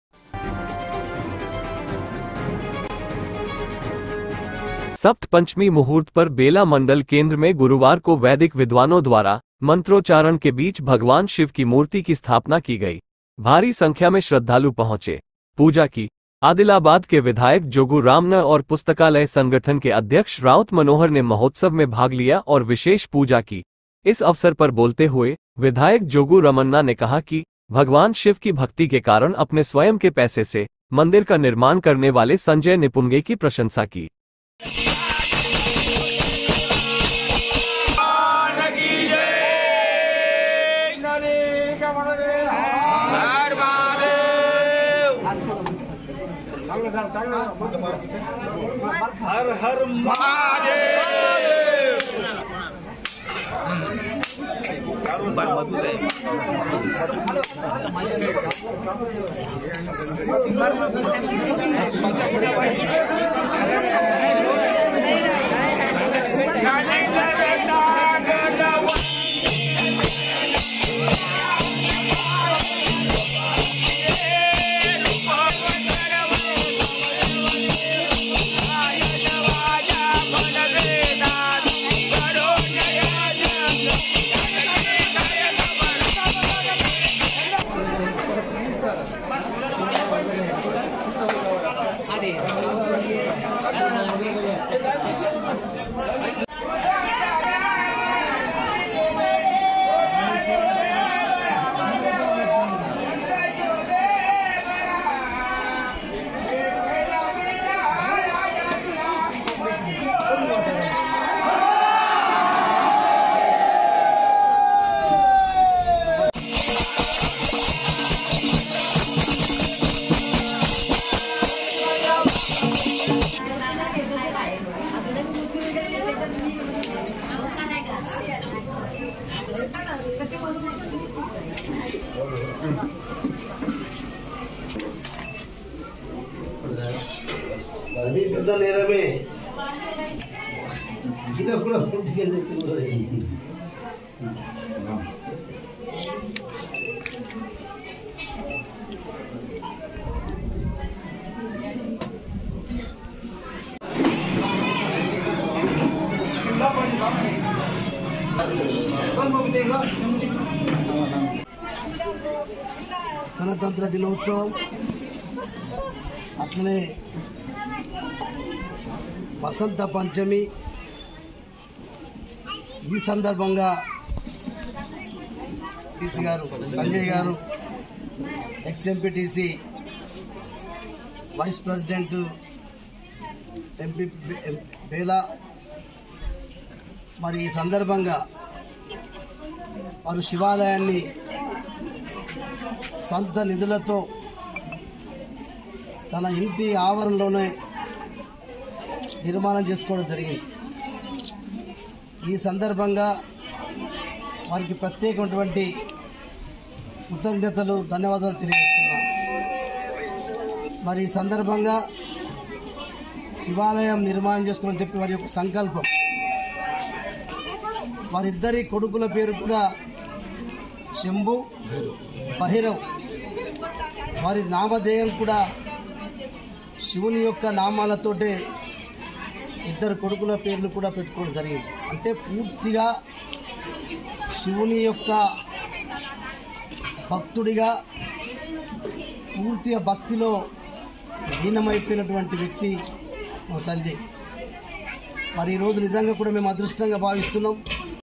bela shahar me bhagwan shiv ki murti ka pratishtapan
सप्त पंचमी मुहूर्त पर बेला मंडल केंद्र में गुरुवार को वैदिक विद्वानों द्वारा मंत्रोच्चारण के बीच भगवान शिव की मूर्ति की स्थापना की गई।भारी संख्या में श्रद्धालु पहुंचे। पूजा की। आदिलाबाद के विधायक जोगू रामन्ना और पुस्तकालय संगठन के अध्यक्ष राउत मनोहर ने महोत्सव में भाग लिया और विशेष पूजा की।इस अवसर पर बोलते हुए, विधायक जोगू रमन्ना ने कहा कि, भगवान शिव की भक्ति के कारण अपने स्वयं के पैसे से, मंदिर का निर्माण करने वाले  संजय निपुंगे की प्रशंसा की।